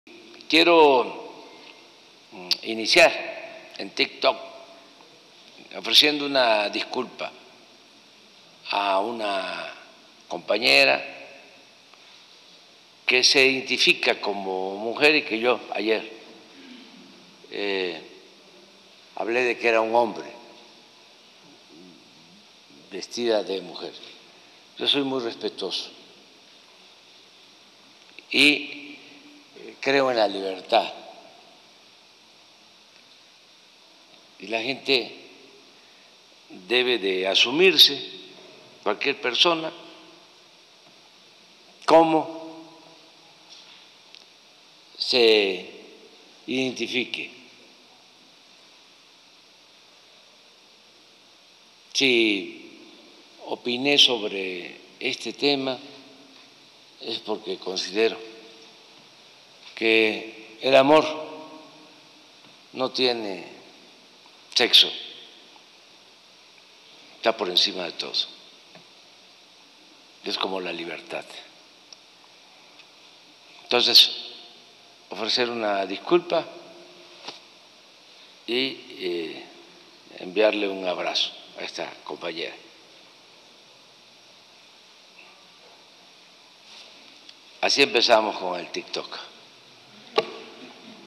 AMLO abrirá su cuenta de TikTok | Iniciará pidiéndole perdón a la diputada trans Salma Luévano
El presidente López Obrador abrirá su cuenta de TikTok y su primera colaboración con la red social no lleva baile pero sí una disculpa a la diputada trans Salma Luévano

“Quiero empezar TikTok con una disculpa a una mujer que yo dije que era hombre. Ofrecer una disculpa a la compañera (…) Si opiné sobre ese tema es porque creo que el amor no tiene sexo, está por encima de eso. Así empezamos con el TikTok”

La MañaneraEnResumen